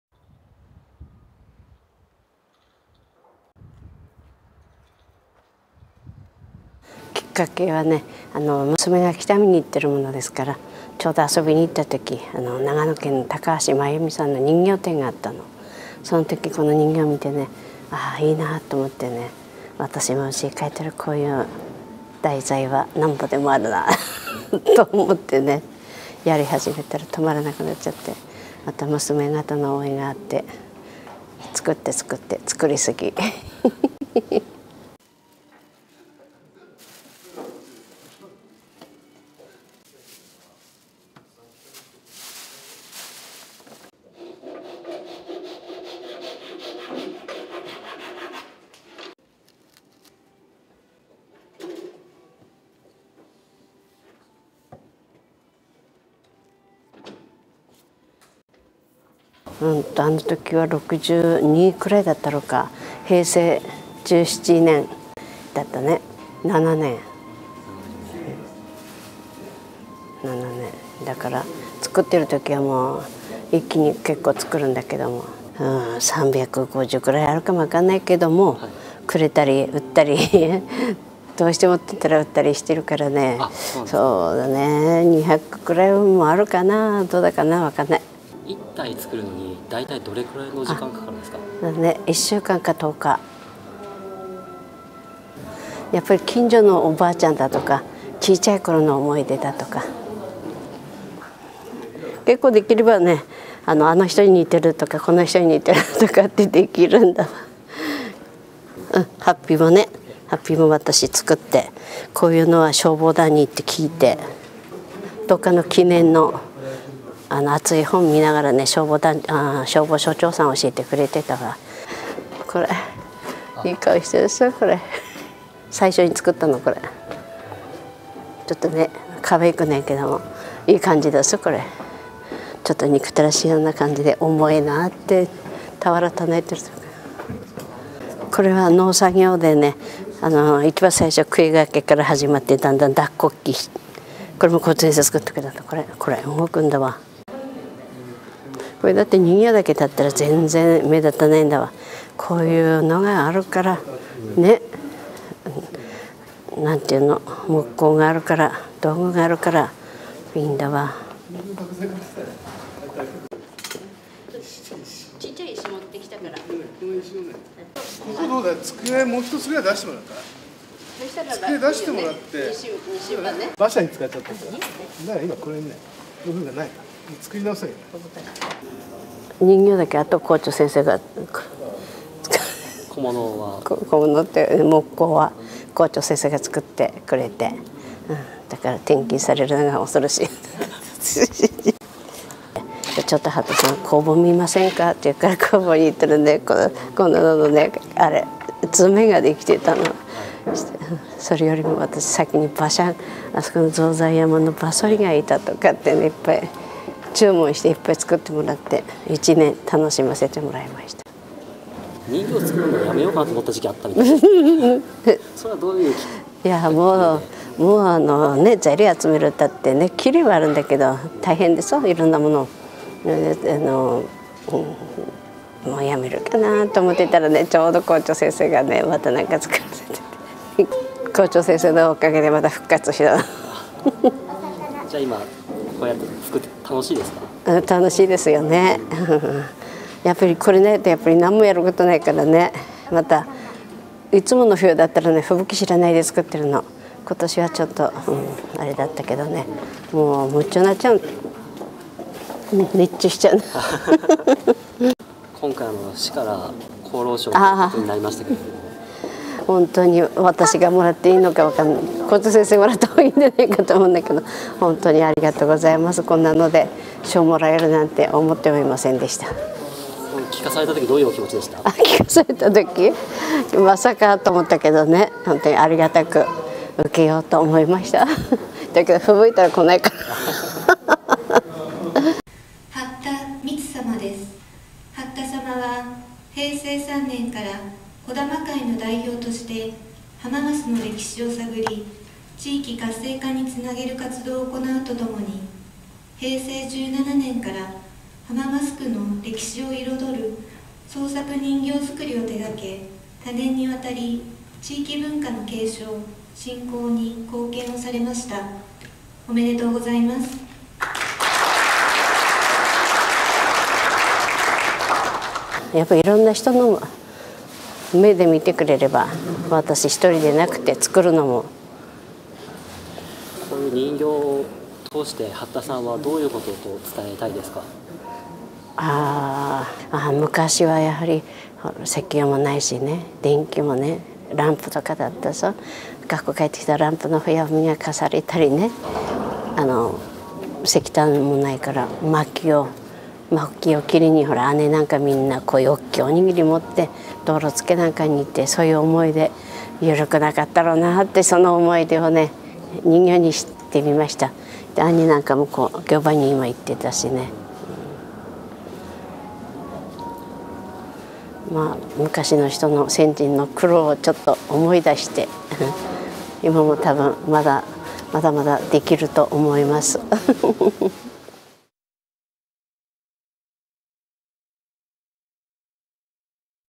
石狩市＠八田美津さん人形展開催 2012/02/22(6分16秒)
えりすいしかりネットテレビ＠1月27日 石狩市民図書館にてインタビュー
八田さんの教育功労章受章をうけて、市民図書館で人形展が開催されました。